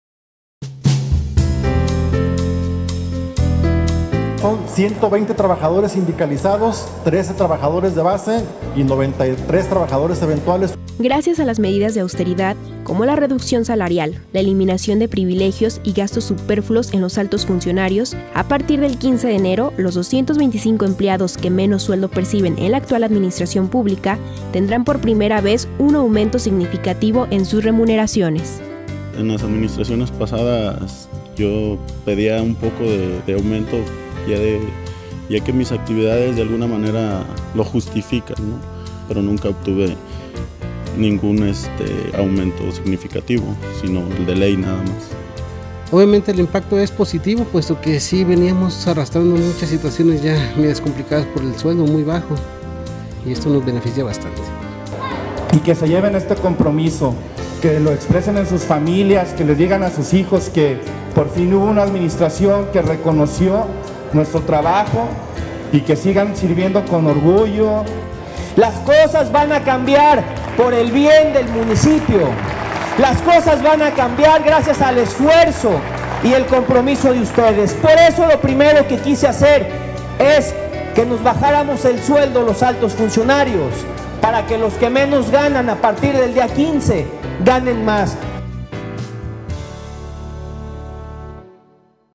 Anuncio Oficial de Nivelación Salarial
Gracias a las medidas de austeridad del gobierno municipal, que van desde la reducción salarial, eliminación de privilegios y gastos superfluos en los altos funcionarios hasta la asignación de un presupuesto ciudadano enfocado a los servicios públicos, se anunció que a partir del 15 de enero, los 225 empleados que menos sueldo perciben en la Administración Pública tendrán, por primera vez, un aumento significativo en sus remuneraciones.

Bajo el lema de “Salarios más juntos para los que menos ganan” y ante la presencia de cientos de trabajadores que se dieron cita en el patio del Palacio Municipal, el presidente municipal, Alberto Esquer Gutiérrez anunció que los trabajadores que ganan una cantidad inferior a los 4000 mil pesos al mes, se beneficiarán de un aumento directo en su sueldo de 1000 pesos mensuales; los empleados públicos que perciban de 4,000 a 5000 pesos verán incrementado su remuneración con 800 pesos mensuales y todos los que se encuentren en un tabulador de 5000 a 6000 pesos al mes, tendrán un aumento directo de 600 pesos.

El primer edil señaló que el incremento salarial es producto de la aprobación de un presupuesto justo y austero consensuado por un gobierno ciudadano donde la gente manda y no tienen privilegios los altos funcionarios. Reiteró que el dinero en Zapotlán el Grande es destinado para los que menos tienen y contrario a lo que sucedía en pasadas administraciones “que sólo se daba un aumento simbólico al salario dependiendo del monto de la inflación, esta vez los trabajadores verán recompensado su esfuerzo de una forma real en sus bolsillos”

Por su parte, los empleados públicos reiteraron su compromiso con la ciudadanía y el gobierno municipal y agradecieron el incremento salarial como un aliciente para dar lo mejor de sí.
José Everardo Espinoza, empleado de Aseo Público comentó que este aumento es el primero que recibe en ocho años y que le ayudará para mejorar la calidad de vida de su familia.

“Así sí dan ganas de trabajar y hacer las cosas mejor cada día porque por fin reconocen todo nuestro esfuerzo”  señaló.